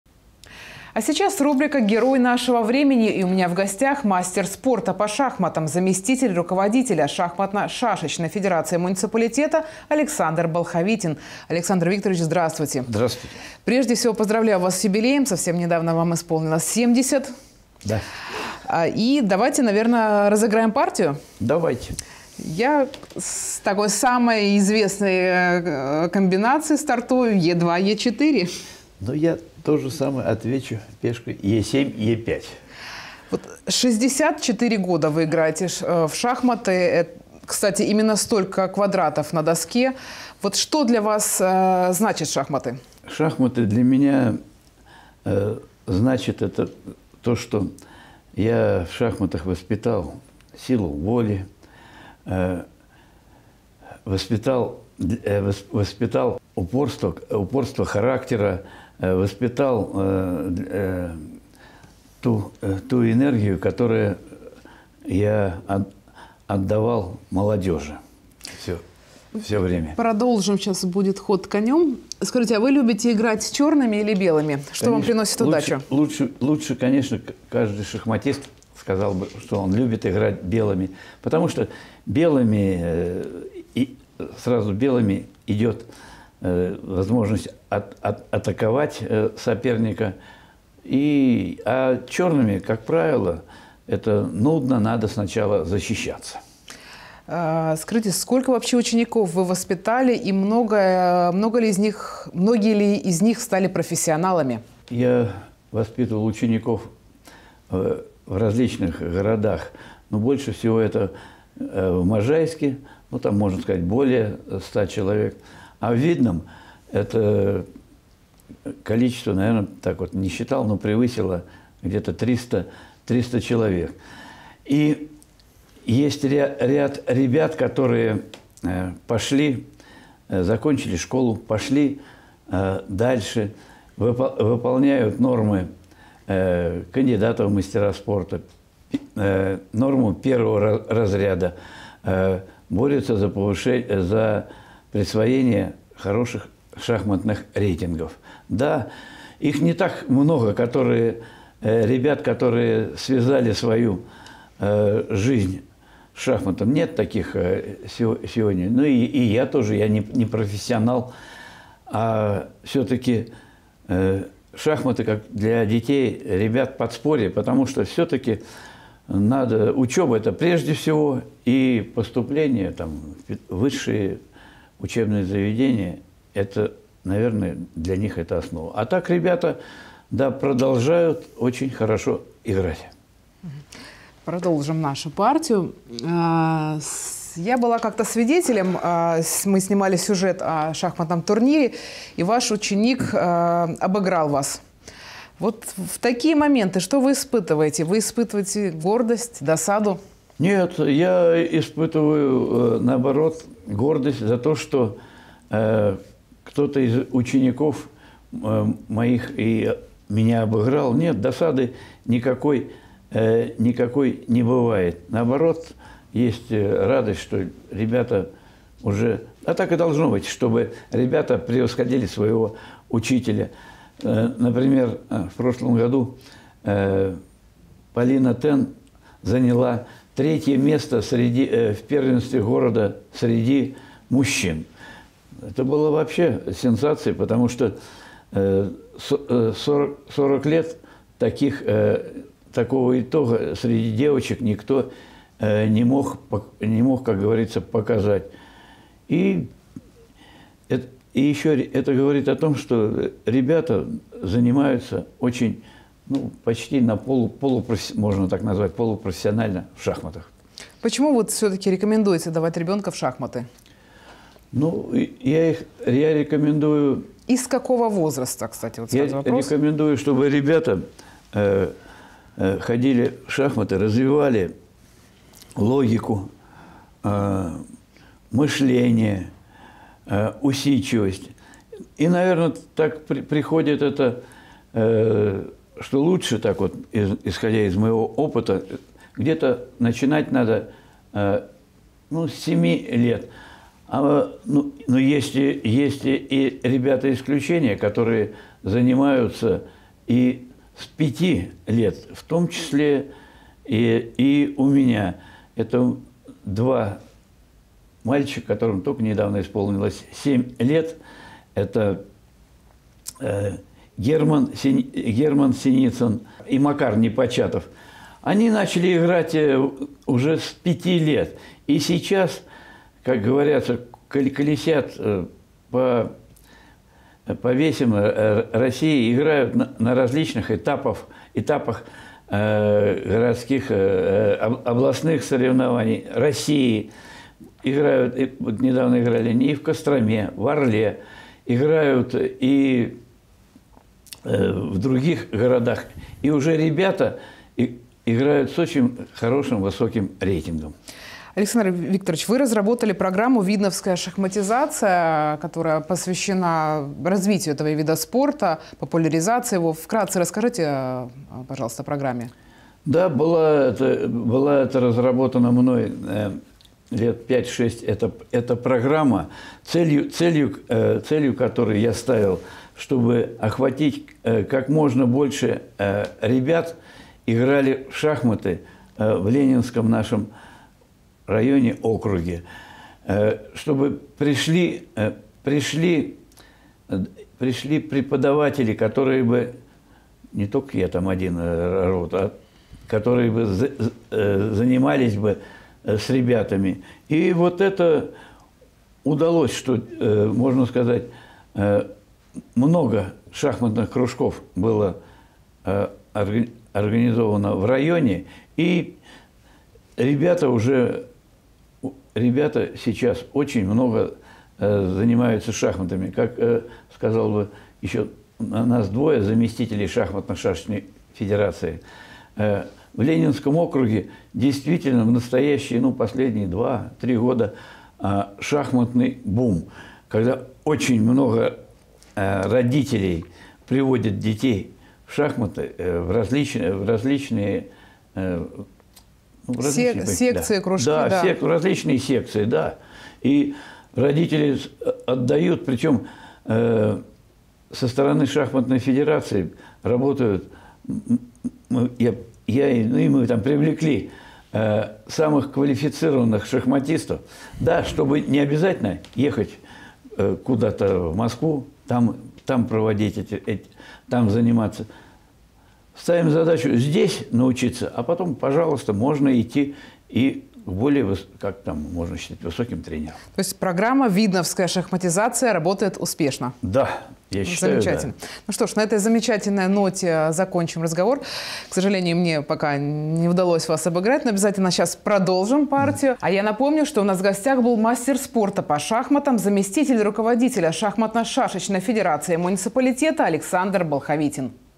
ГЕРОЙ НАШЕГО ВРЕМЕНИ
Рубрику "Герой нашего времени" сегодня представляет мастер спорта по шахматам Александр Болховитин, который недавно отметил 70-летие.